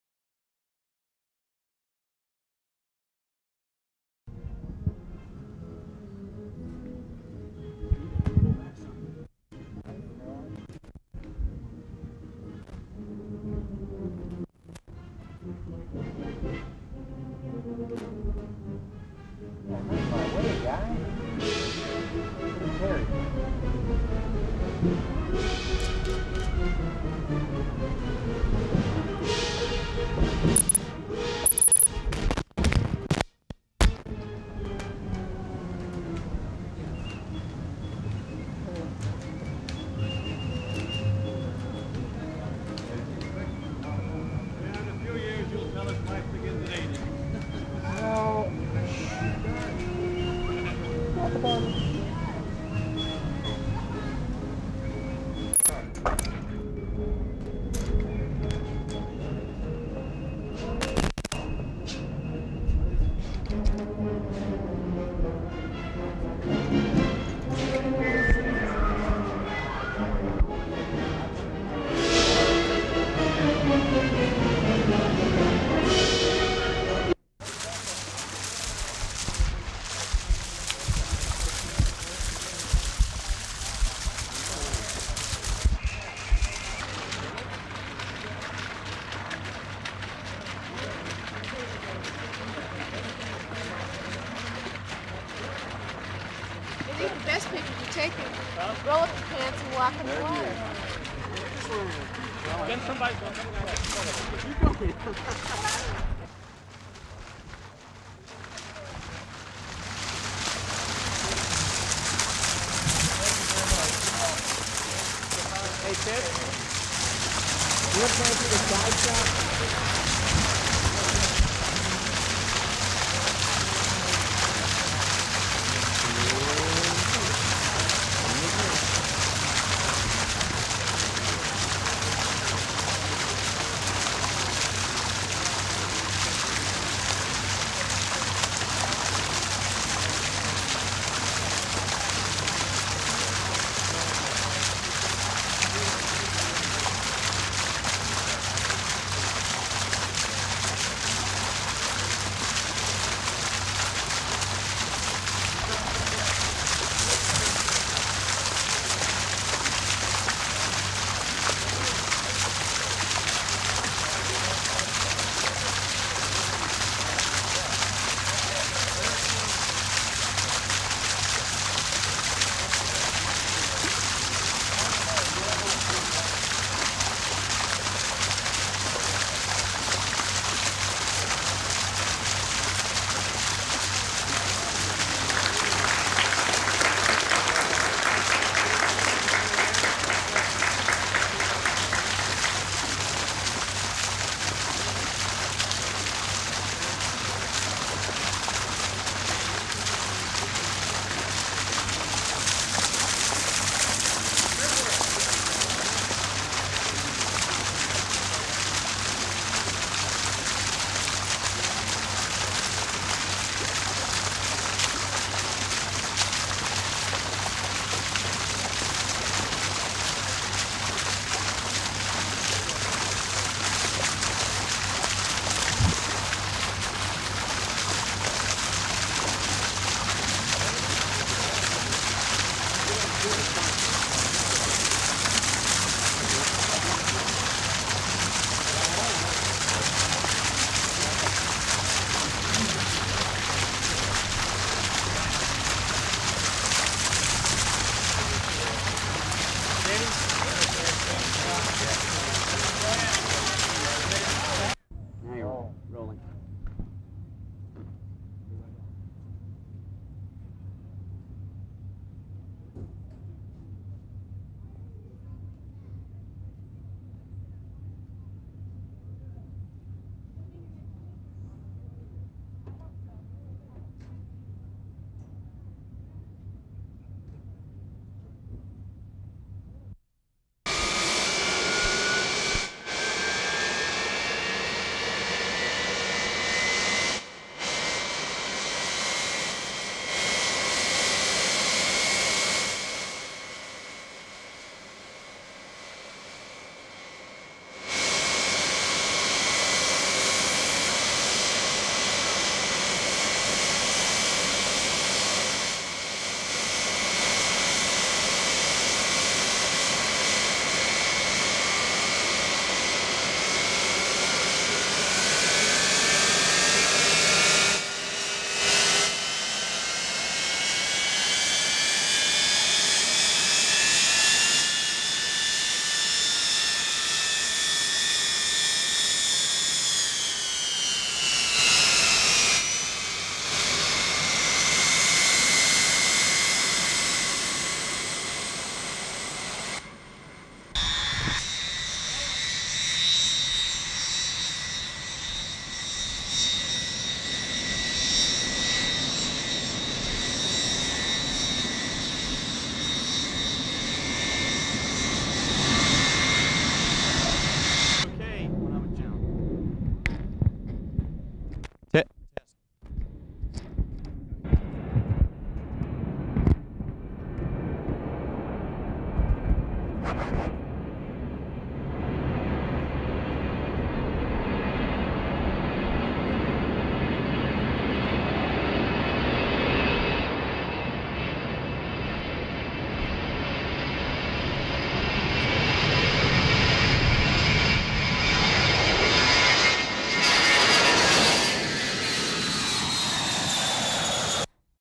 President Reagan at the Jimmy Carter Presidential Library on October 1, 1986
Full Title: Trip to Georgia. President and Nancy Reagan Walking with President Jimmy and Rosalyn Carter Toward Library, Ribbon Cutting Ceremony. Jimmy Carter Presidential Library, President and Nancy Reagan Depart on Air Force One in Atlanta on October 1, 1986

Transcript: N/A

Production Date: 10/1/1986

Access Restriction(s):Unrestricted
Use Restriction(s):Unrestricted